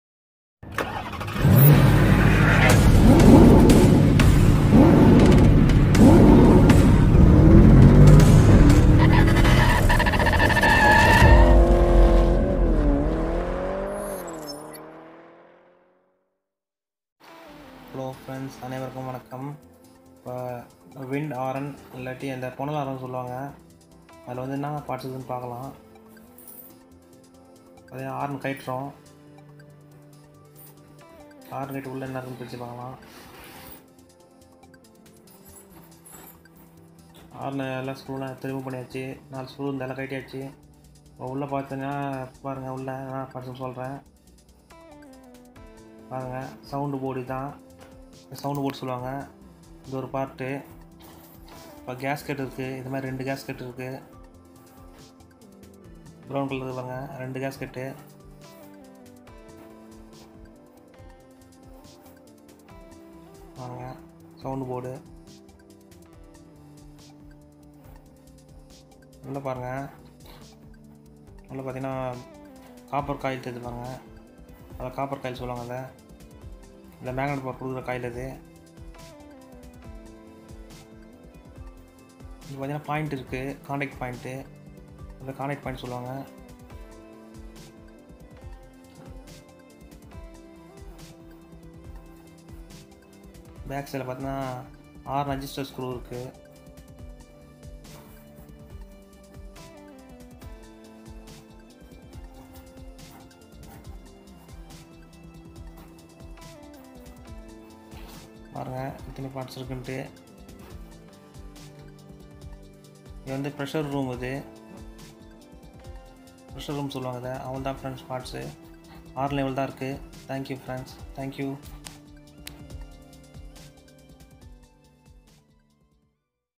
what is inside the wind tone horn in tamil
This video is what is inside the wind tone horn and removed parts